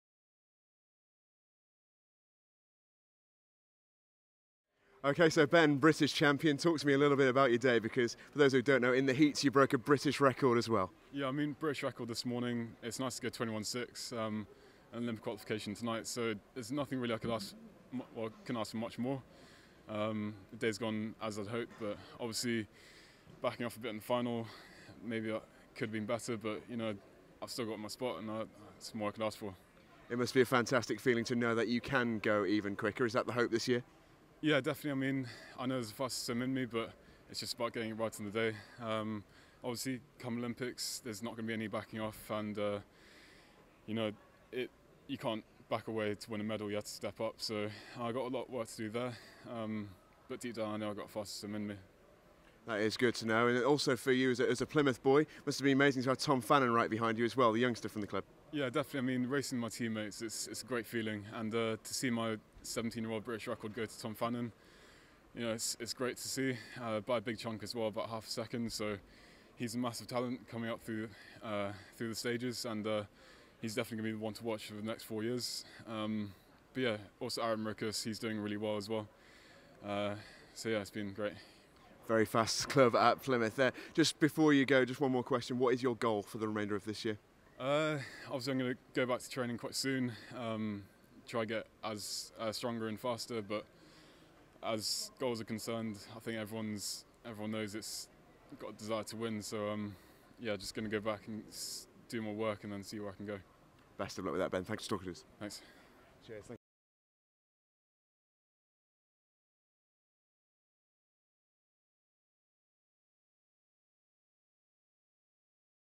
Ben Proud - British Champion 2016 - Men's 50m Freestyle
A delighted Ben Proud added to his British Record in the heats by claiming Gold in the final of the men's 50m freestyle